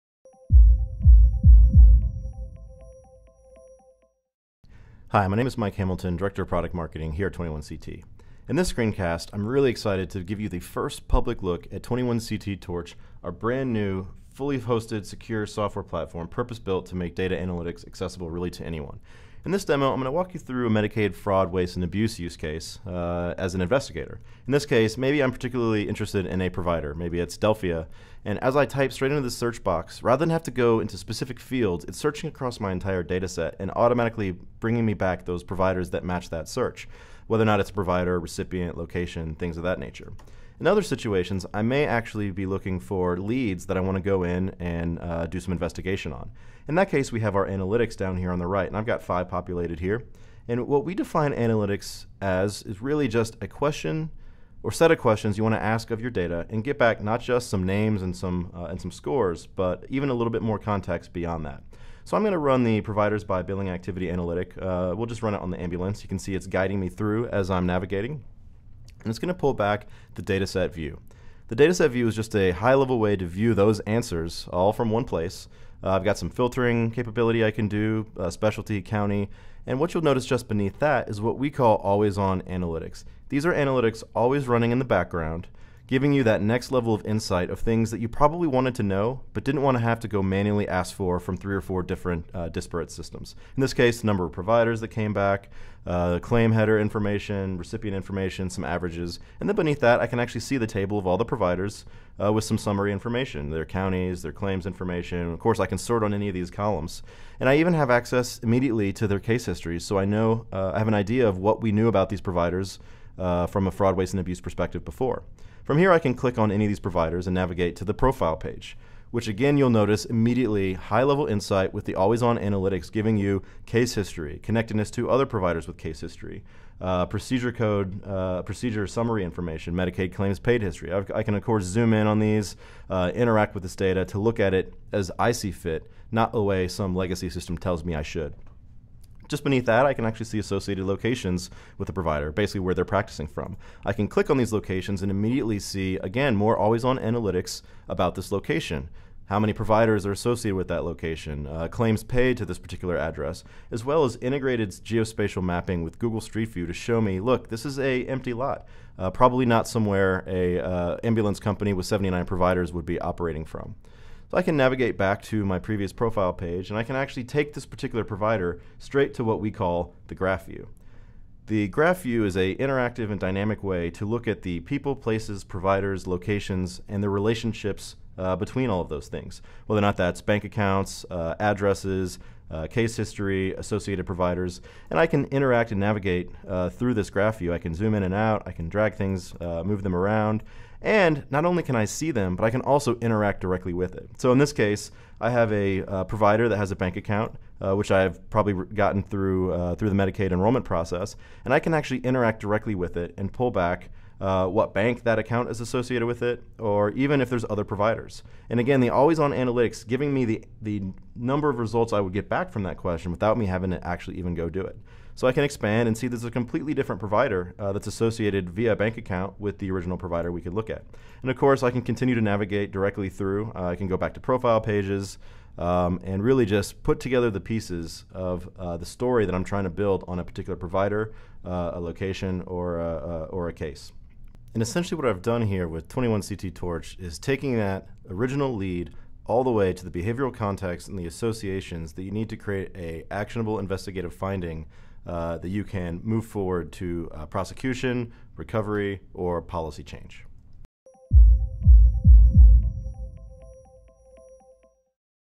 21CT Torch | Medicaid Fraud Analytics
21CT Torch℠, the next generation of LYNXeon, optimized for fraud detection and investigative analytics, is the result of 15 years working with investigators of every different skill level. The system rewards human curiosity with every click by being intuitive, adaptive, and always-on. Torch provides investigators with multiple perspectives, viewpoints, and signposts to help build the case, and also the computational horsepower and automated data knowledge delivery to do it all quickly. This unique combination has made it the ideal platform to fight the healthcare fraud epidemic.